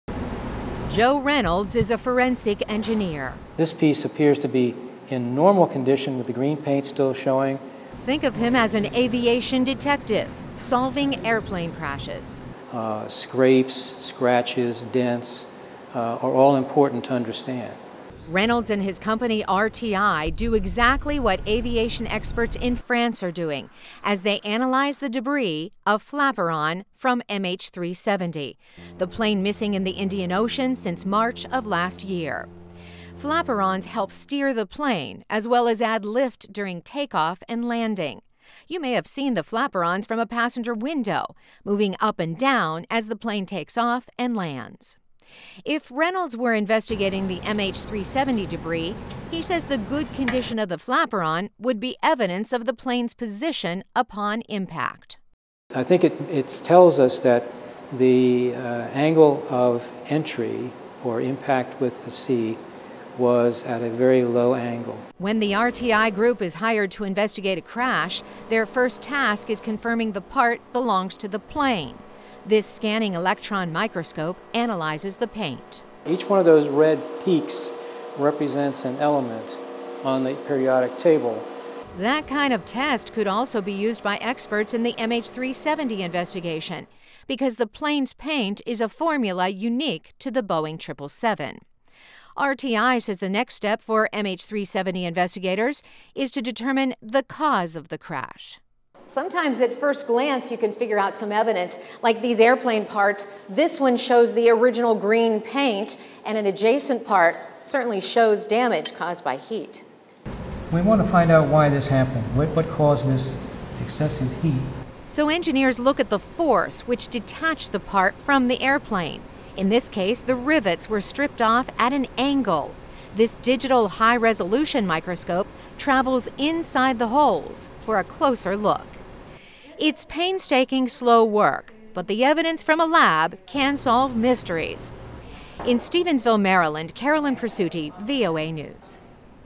Forensic Engineers Show How Plane Crashes Are Investigated
Aviation experts at a laboratory in France continue to examine the piece of debris from Malaysia Airlines flight 370 that washed up on the shores of Reunion Island in the western part of the Indian Ocean. The investigation involves searching for clues with the naked eye — and under powerful microscopes. VOA's Carolyn Presutti visited a forensic laboratory in the U.S. to show how it's done.